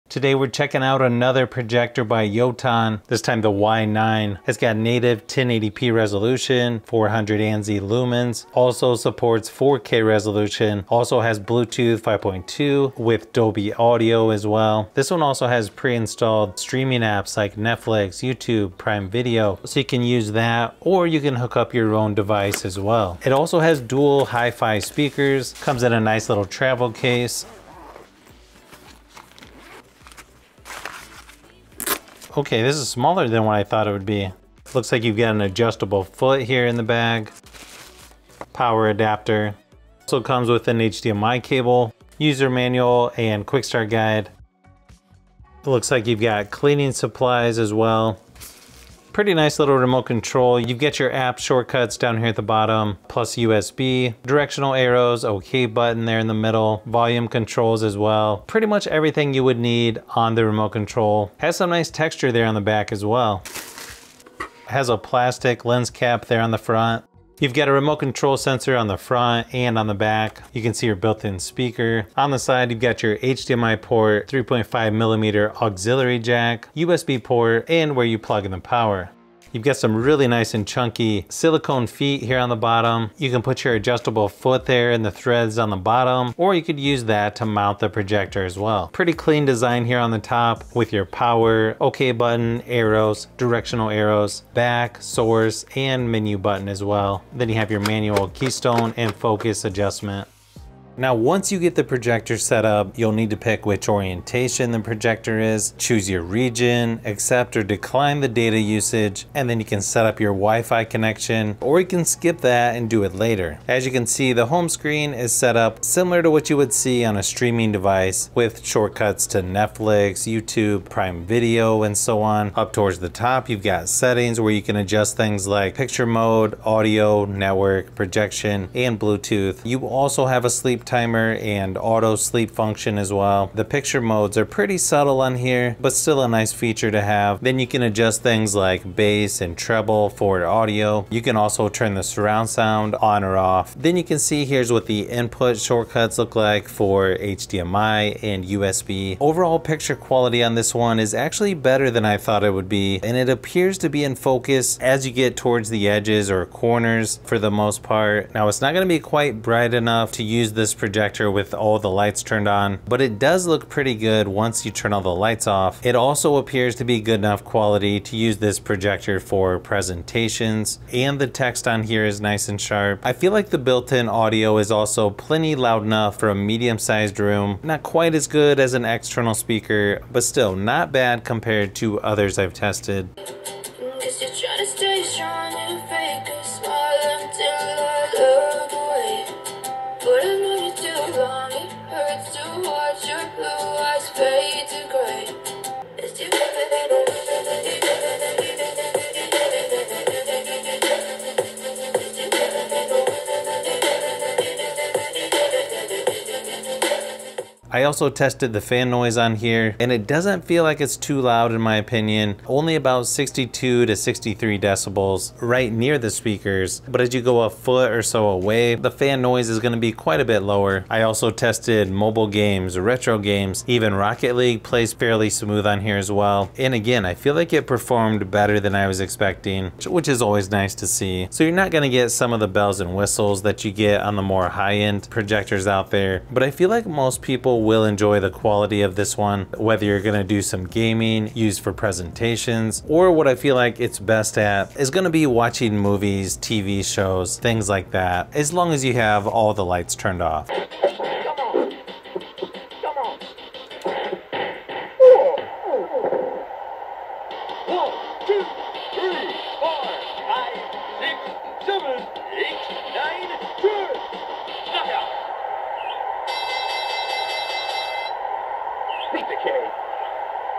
$159 Netflix/YouTube Certified Projector - Is It Worth It? (Yoton Y9)
Yoton Y9 Mini Projector on Amazon

This projector comes with Netflix, Prime Video, YouTube and other apps built-in. This smart WiFi Projector has 400 ANSI Lumen Native 1080P, works with your phone for home movie setup